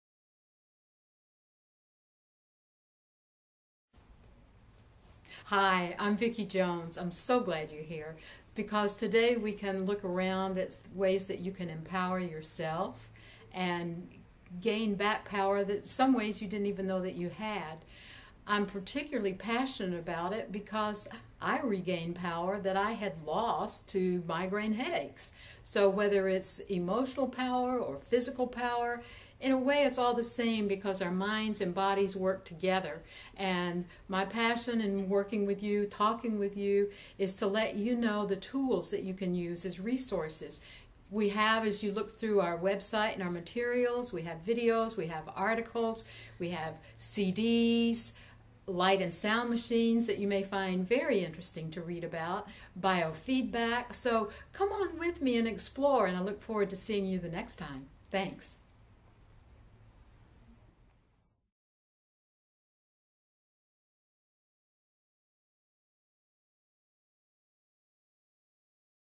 Introducing Vicki Jones from Smarterway.com
Vicki Jones LCSW introduces Biofeedback Therapy. Biofeedback Therapy is a training technique in which you learn to improve your health and performance by using signals from your own body.